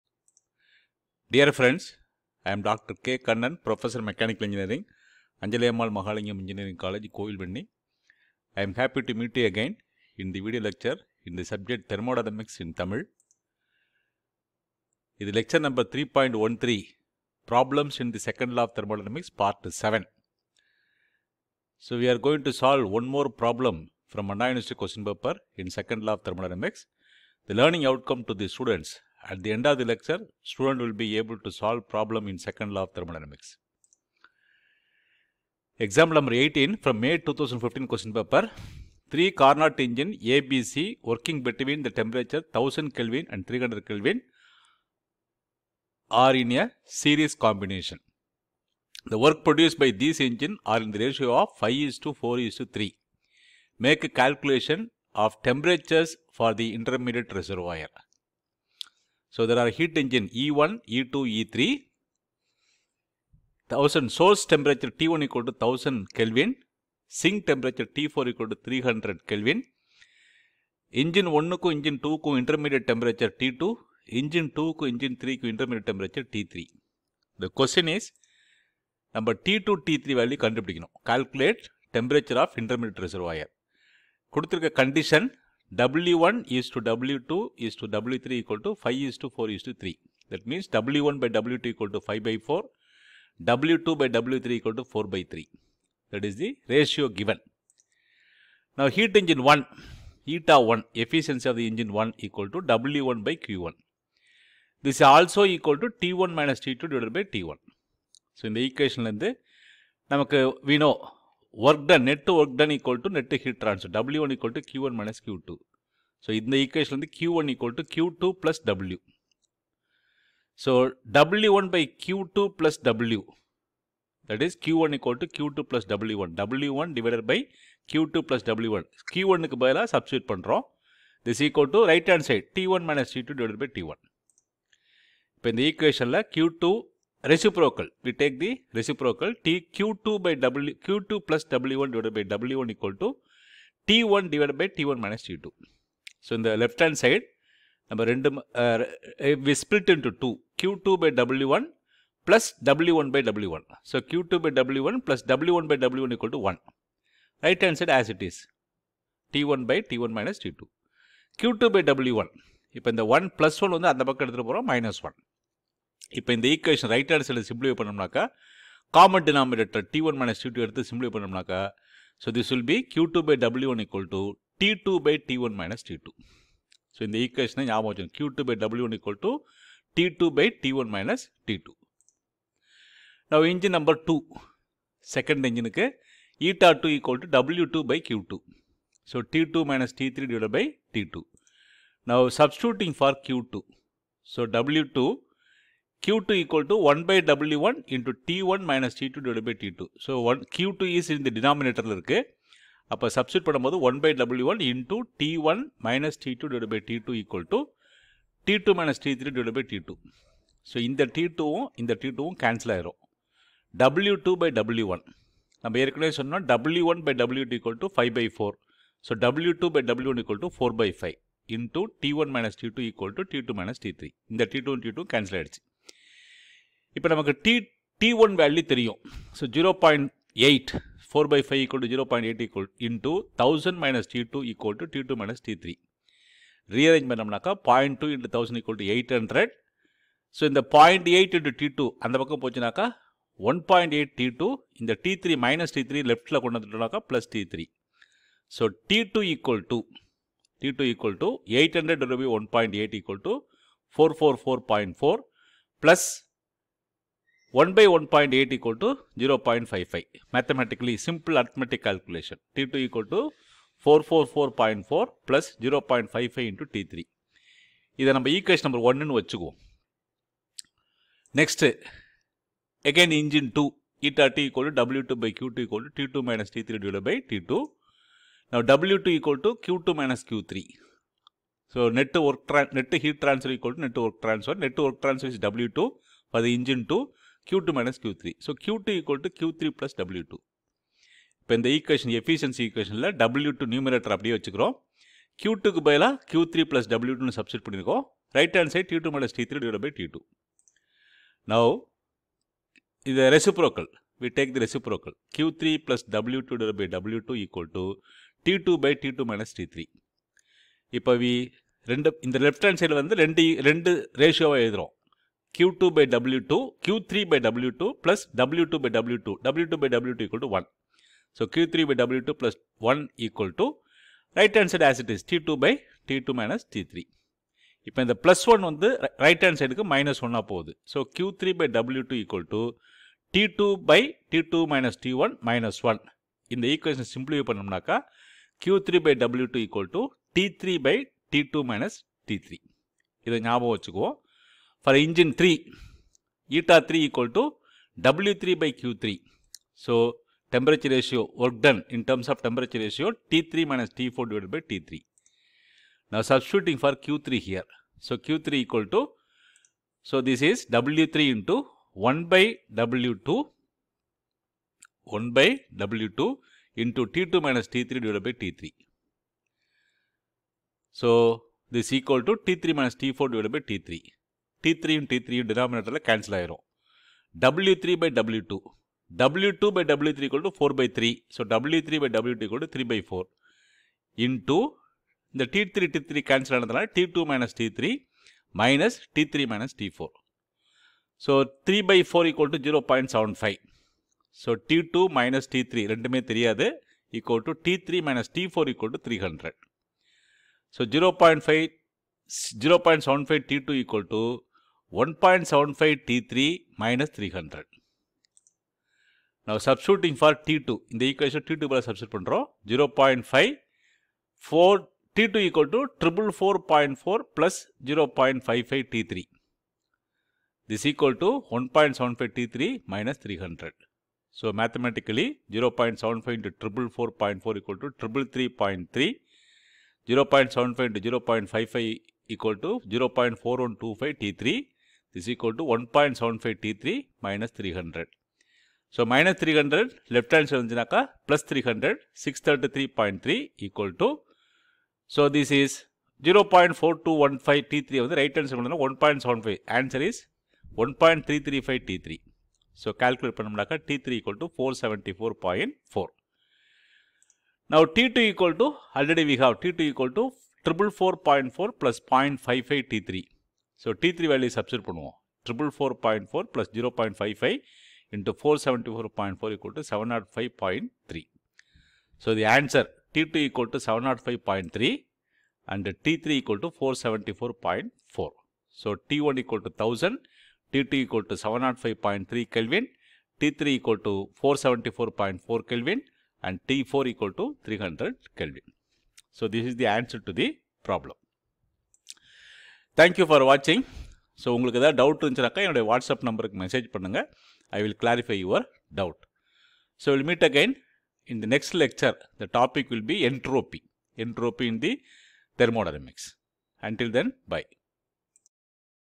TD 3.13 Problems in II Law of Thermodynamics - 7
Problem solving from Anna University exam question paper in second law of thermodynamics. heat engines in series